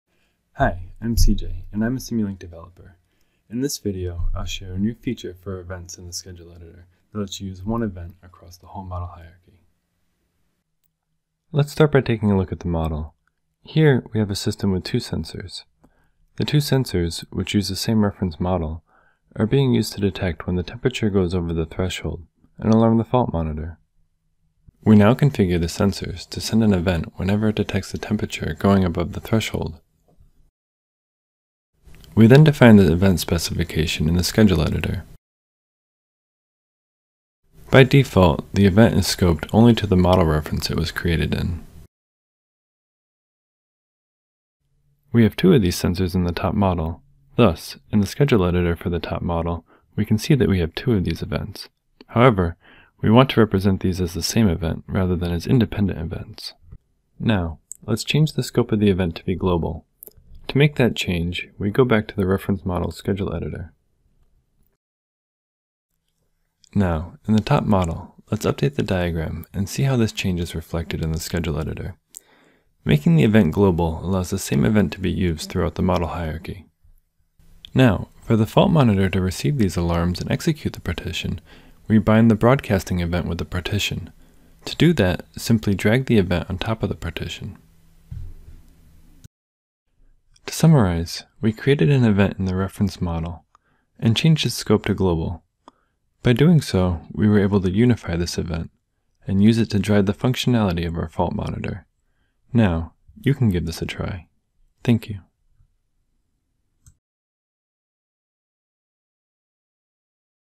How to Schedule a Model with Global Events
Learn how to configure the scope of an event in Schedule Editor and use a global event to drive execution throughout the model hierarchy.